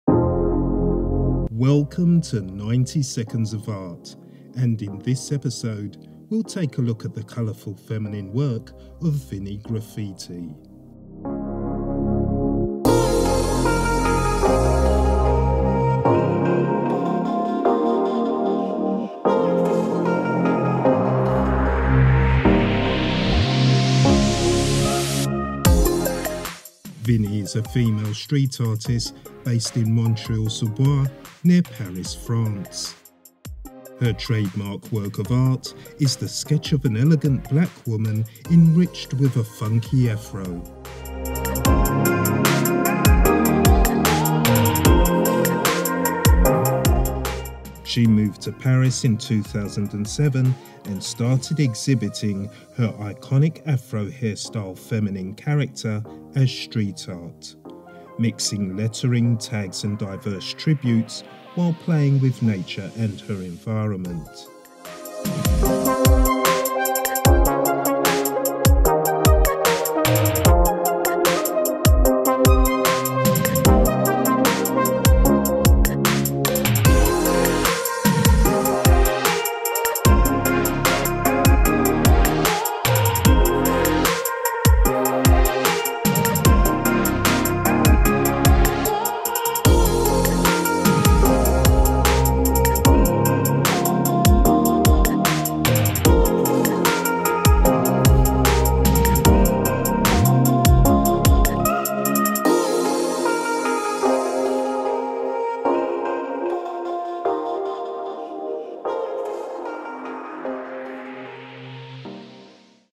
Street Art by Vinie Graffiti
Welcome to 90 seconds of art, I hope you enjoyed the next minute and a half of some great Street Art from Vinie Graffiti ,if you're not familiar with her work this video is a great introduction into her art.

Narrator:Steve